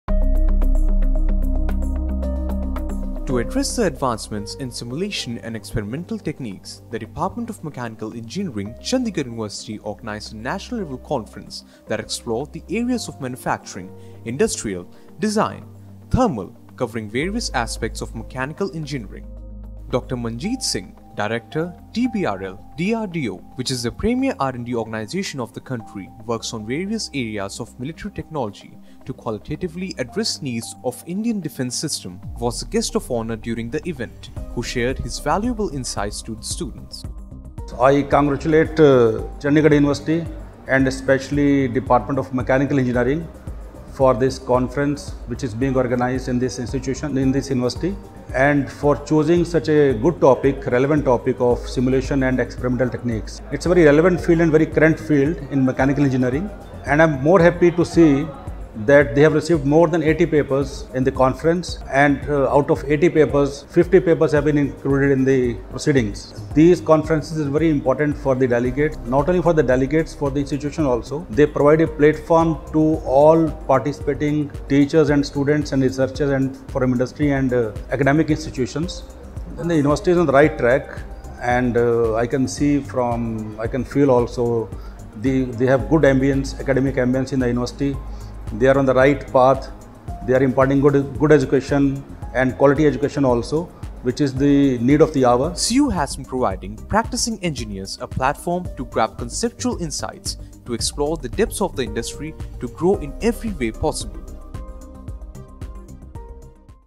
NCASEme 2019 | Mechanical Engineering | Chandigarh University
6th National Conference on Advancements in Simulation & Experimental Techniques in MechanicalEngineering

Department of Mechanical Engineering, CU organized a National Conference on Advancements in Simulation & Experimental Techniques in Mechanical Engineering (NCASEme-2019). Dr. Manjit Singh, Director TBRL, DRDO who was the guest of honor, presented the Keynote Address on “Advancements in Simulation and Experimental Techniques in the area of Manufacturing, Industrial, Design, Thermal and other areas related to mechanical engineering”. More than 60 research papers were presented at the conference by the delegates who came across from various reputed institutes of India. NCASEme provided an excellent platform to the researchers, scientists and practicing engineers to share their experiences & ideas.